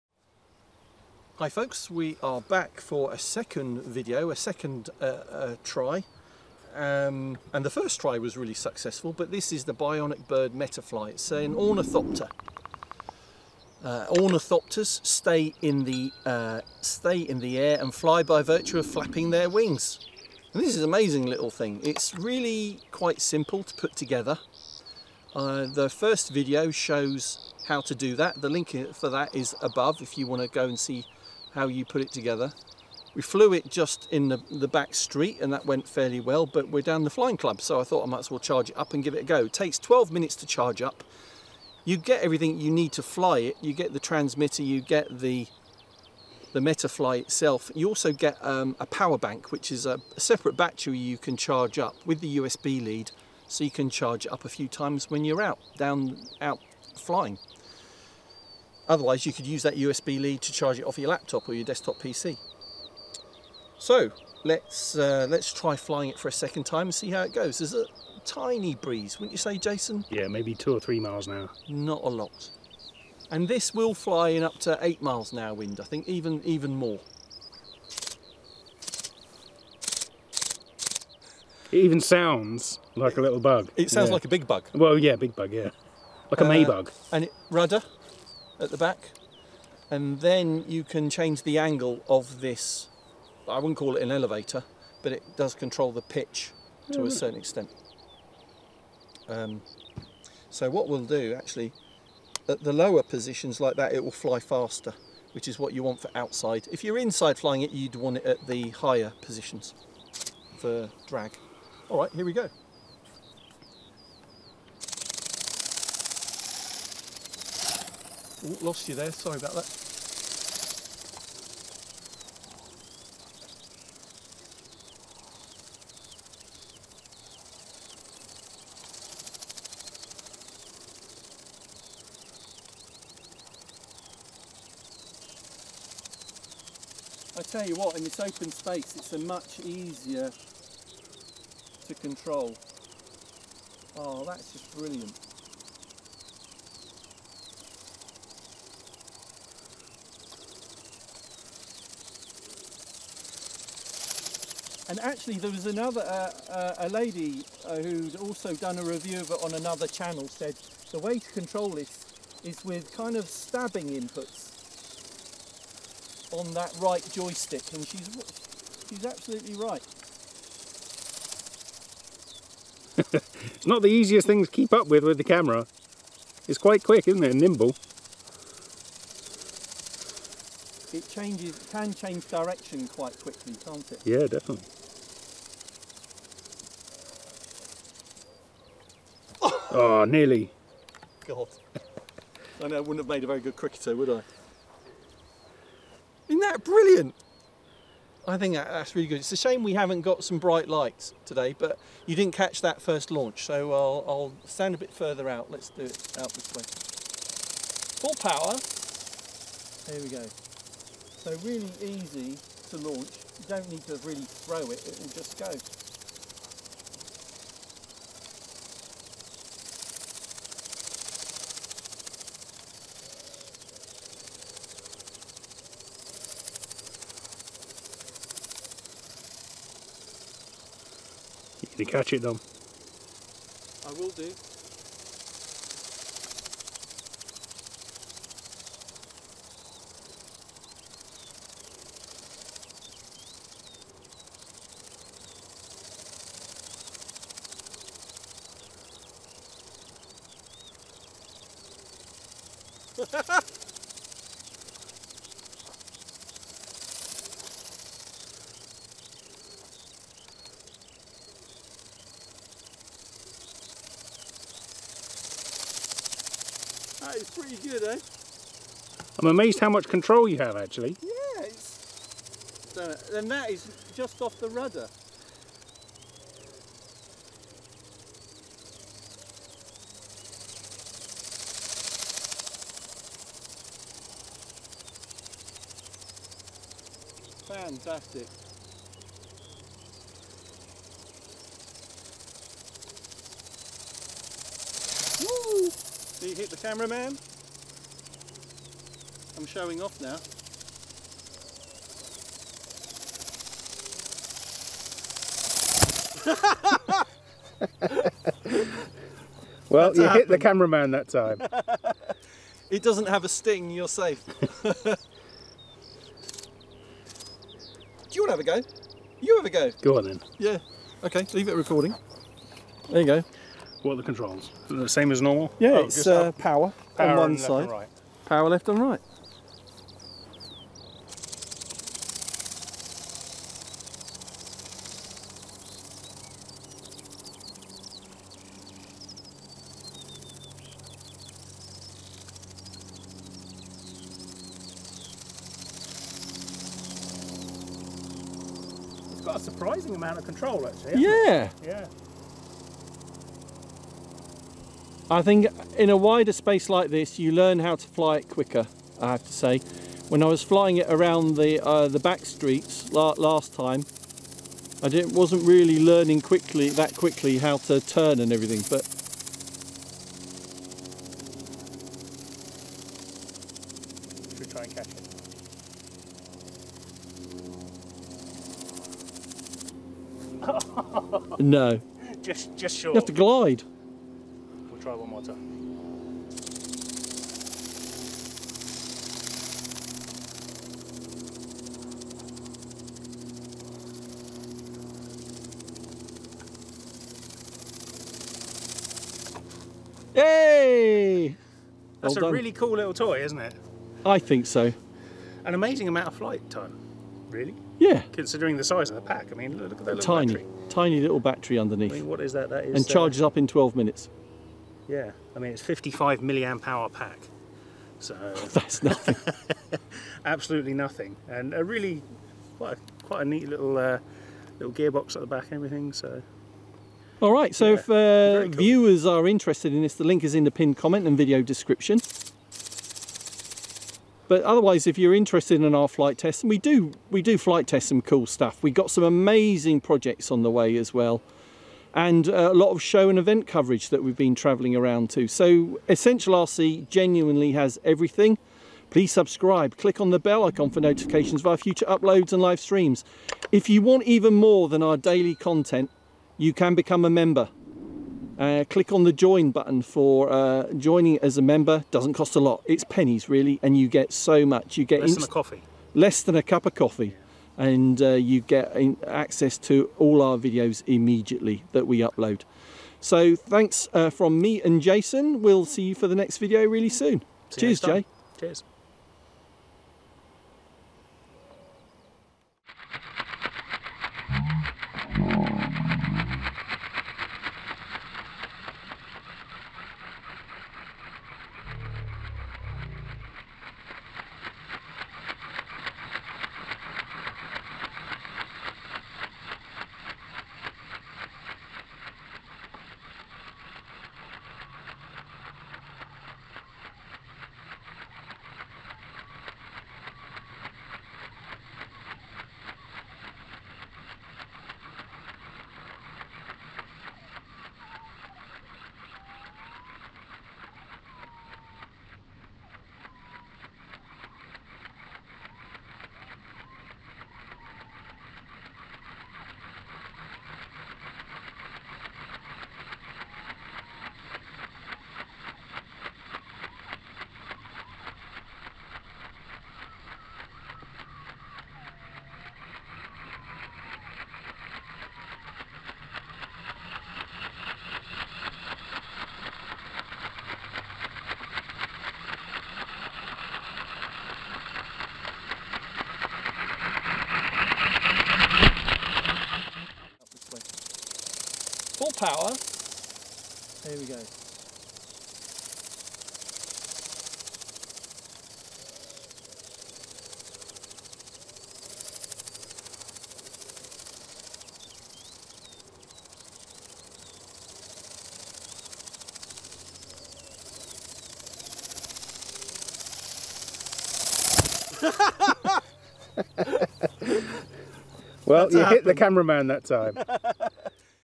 BIG REMOTE CONTROLLED FLYING BUG | bionicbird MetaFly | EASY TO FLY !
Follow my guidance in my previous video and assembling this MetaFly ornithopter is very quick and easy. Then put AAs in the controller/transmitter and charge up the MetaFly with the provided USB charge lead into your laptop or mains supply with a USB converter. Also charge up the provided power bank so you can charge up the MetaFly when out flying. We had to manually bind the MetaFly to the controller/transmitter but this was easy following the instructions in the manual.

Flying the MetaFly smoothly takes a little practice but I got the hang of it after 3 or 4 attempts. It's a very light and robust little flying creature so you are highly unlikely to damage it should you collide with the ground or any other object. Lots of fun for kids and adults !

Filmed by Dom and Harry for the 'Essential RC' YouTube channel using the Panasonic AG-UX180 Pro 4K camcorder.